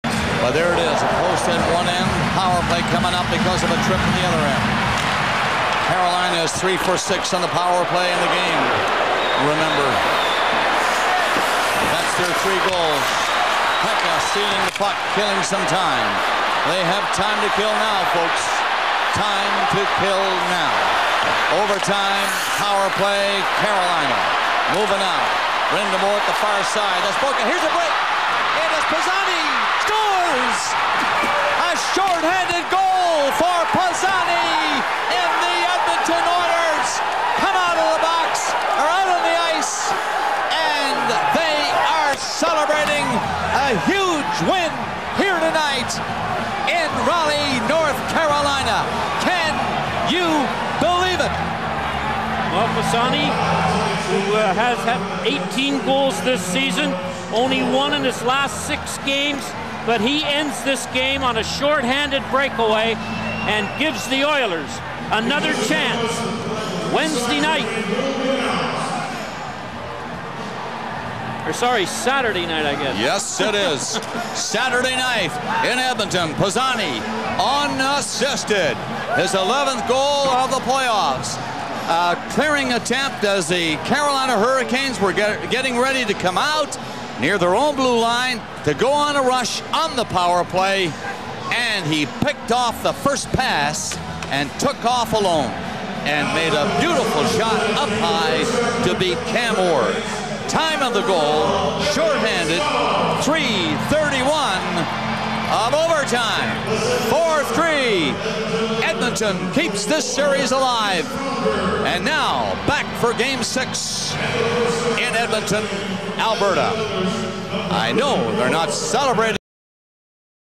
Fernando Pisani's Game 5 OT Winner
Edmonton Oilers fans have a local hero in Fernando Pisani, whose shorthanded overtime winner in Game 5 of the Stanley Cup Finals brings the series back to Rexall Place.